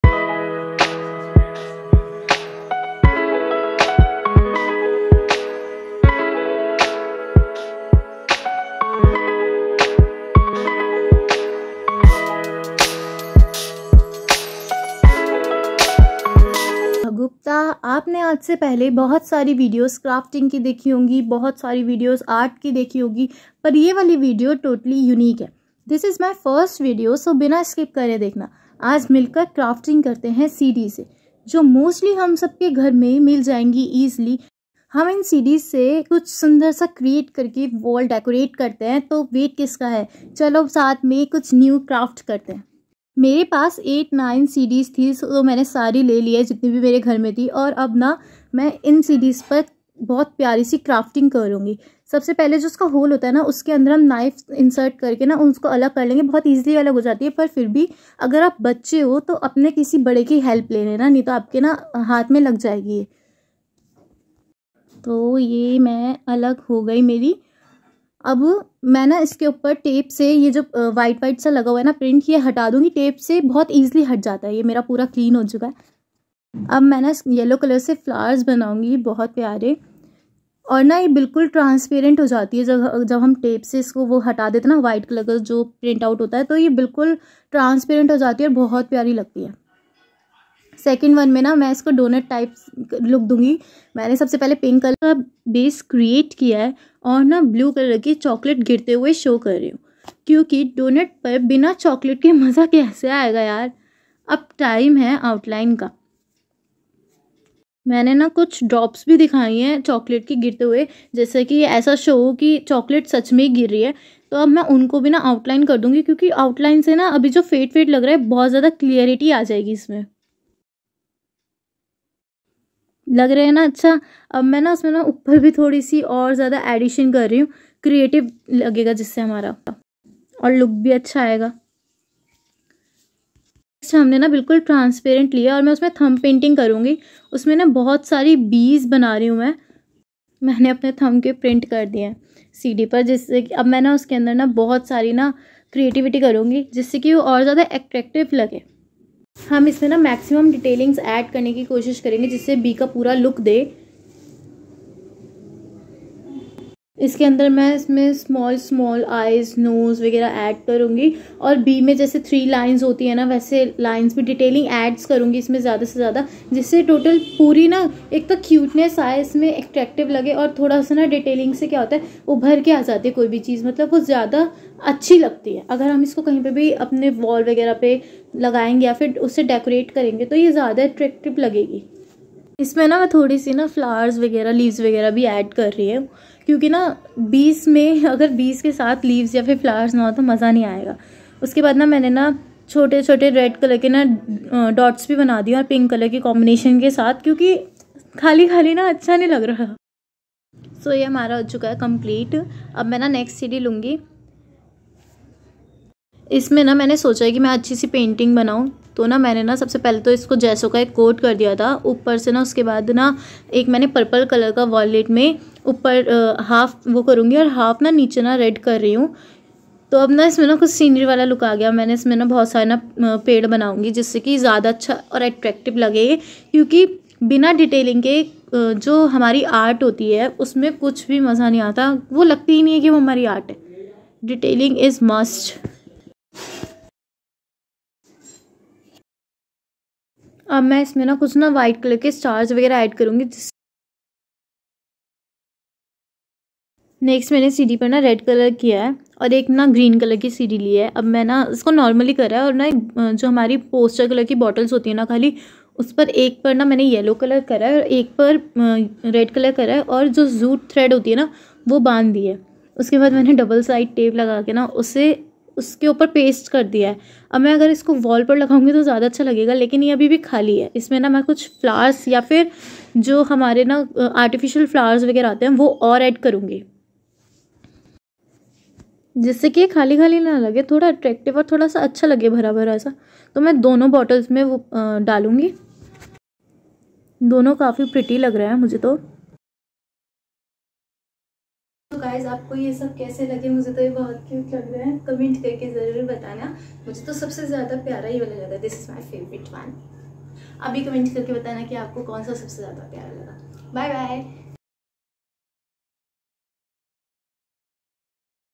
painting on CD | Homedecore | Easy art | Megha gupta💜
In this vedioe I am crafting wall decore with CD.
I hope you'll enjoy this vedio and get some idea to created things with waste CD.